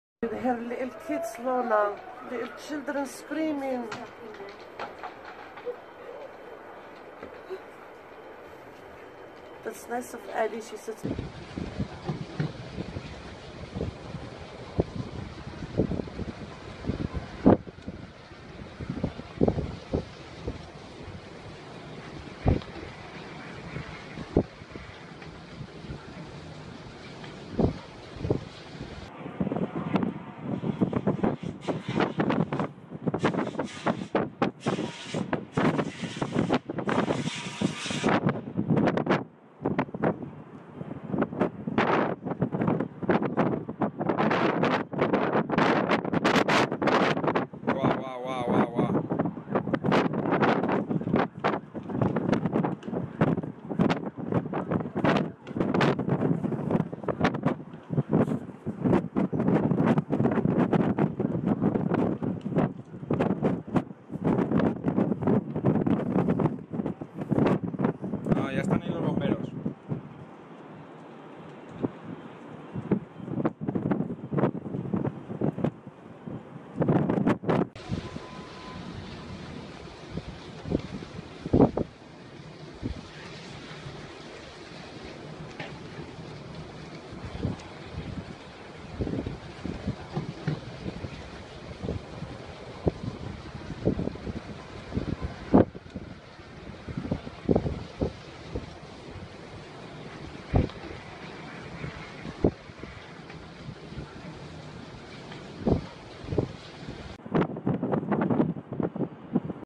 Giant fire engulfs 24-storey London apartment - Grenfell Tower
LONDON: A massive fire ripped through a 27-storey apartment block in west London before dawn on Wednesday, killing several people and injuring dozens more after residents were left trapped inside.

Deaths Reported In London Tower Fire, Eyewitnesses Say People Fell Out.
There have been a number of fatalities in the London Grenfell Tower fire, chief Dany Cotton told reporters at the scene.

Video from Mail Online and other Creative common sources